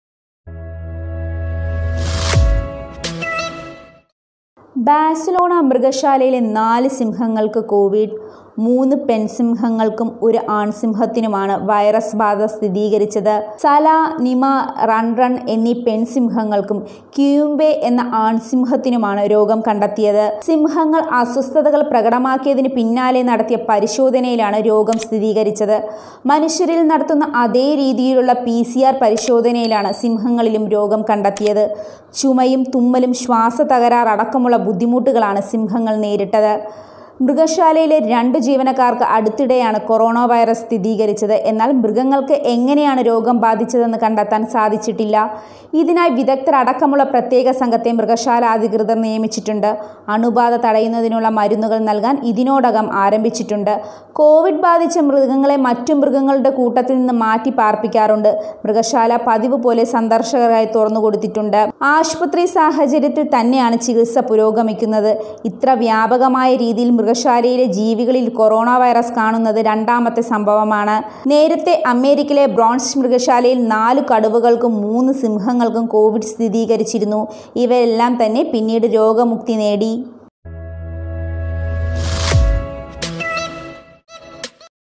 മൃഗശാലയിലെ സിംഹങ്ങൾക്ക് കോവിഡ്| four lions tested positive for Covid 19
ബാഴ്‌സലോണ മൃഗശാലയിലെ നാല് സിംഹങ്ങള്‍ക്ക് കോവിഡ്. മൂന്ന് പെണ്‍ സിംഹങ്ങള്‍ക്കും ഒരു ആണ്‍ സിംഹത്തിനുമാണ് വൈറസ് ബാധ സ്ഥിരീകരിച്ചത്. സല, നിമ, റണ്‍ റണ്‍ എന്നീ പെണ്‍ സിംഹങ്ങള്‍ക്കും കിയുമ്പെ എന്ന ആണ്‍ സിംഹത്തിനുമാണ് രോഗം കണ്ടെത്തിയത്.